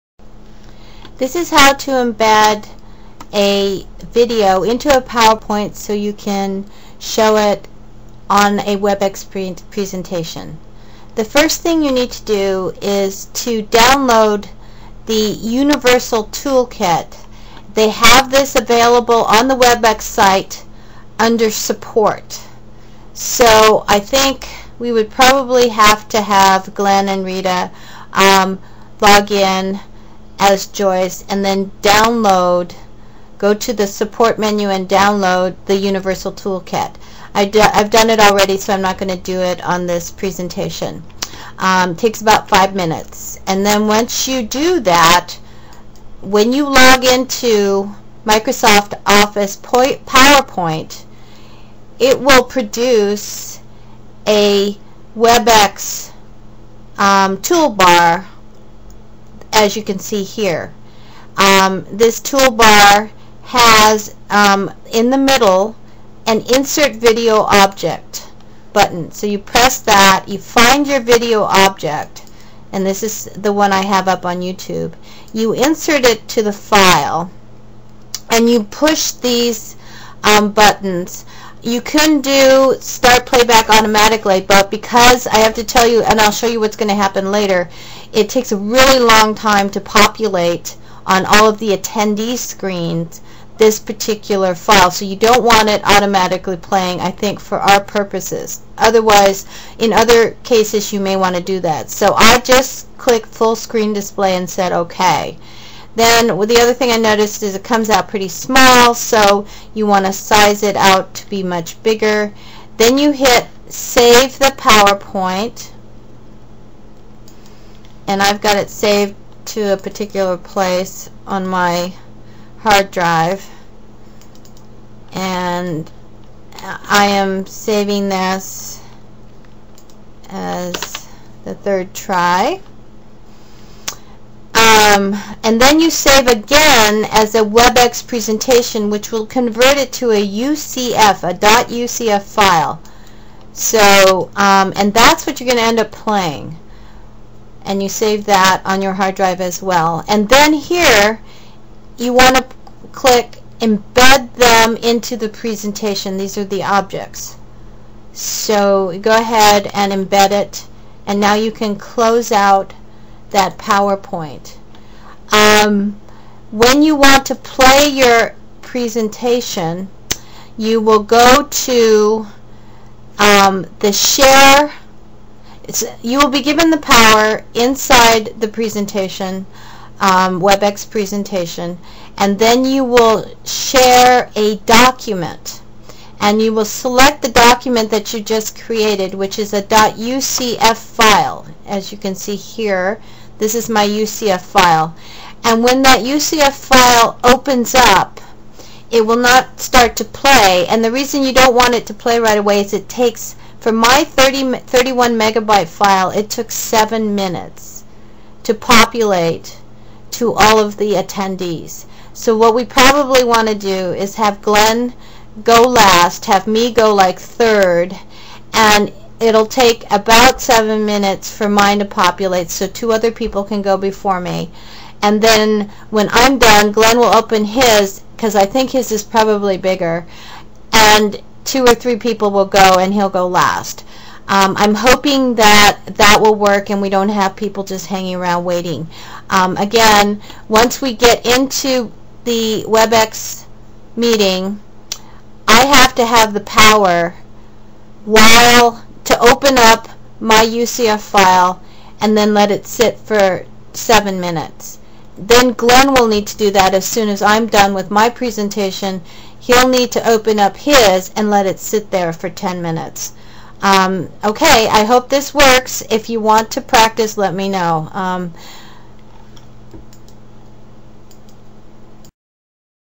How to Embed Video into Powerpoint for a Webex Meeting
I learned now to do this today.  Webex does not post the instructions because they are TOO COMPLICATED.  This is about 4 minutes.